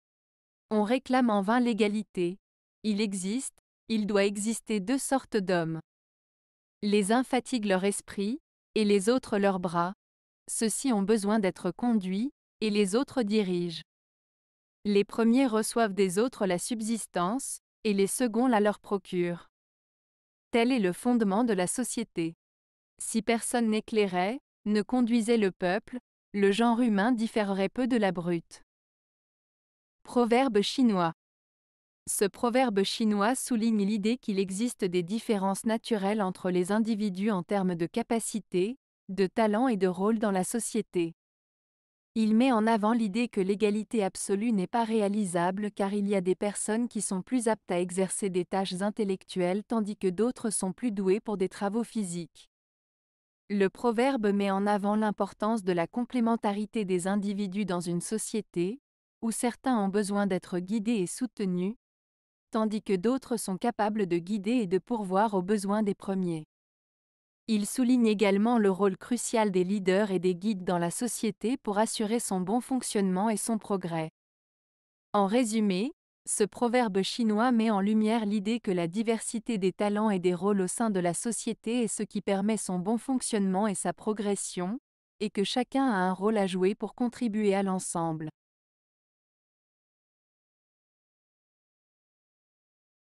Proverbe Chinois - On réclame en vain l'égalité . il existe, il doit exister deux sortes d'hommes. L
Ce proverbe chinois souligne l'idée qu'il existe des différences naturelles entre les individus en termes de capacités, de talents et de rôles dans la société. Il met en avant l'idée que l'égalité absolue n'est pas réalisable car il y a des personnes qui sont plus aptes à exercer des tâches intellectuelles tandis que d'autres sont plus douées pour des travaux physiques.

Le proverbe met en avant l'importance de la complémentarité des individus dans une société, où certains ont besoin d'être guidés et soutenus, tandis que d'autres sont capables de guider et de pourvoir aux besoins des premiers. Il souligne également le rôle crucial des leaders et des guides dans la société pour assurer son bon fonctionnement et son progrès.

En résumé, ce proverbe chinois met en lumière l'idée que la diversité des talents et des rôles au sein de la société est ce qui permet son bon fonctionnement et sa progression, et que chacun a un rôle à jouer pour contribuer à l'ensemble.